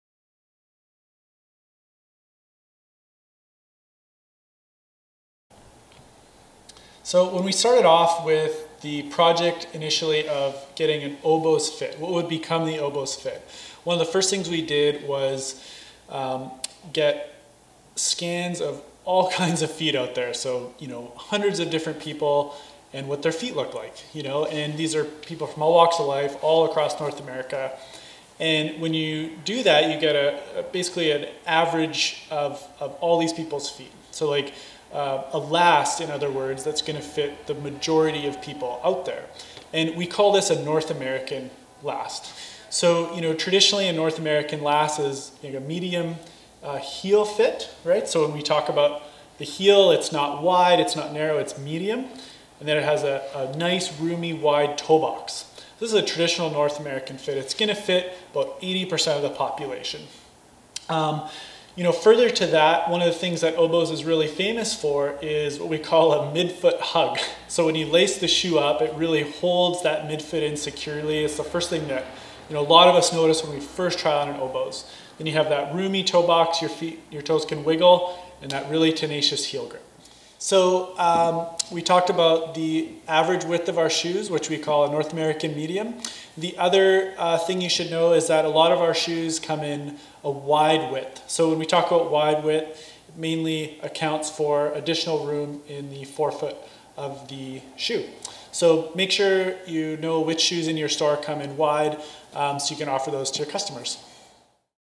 Oboz - Fit
Ross goes over how an Oboz shoe fits, and the characteristics you'll find in all Oboz footwear.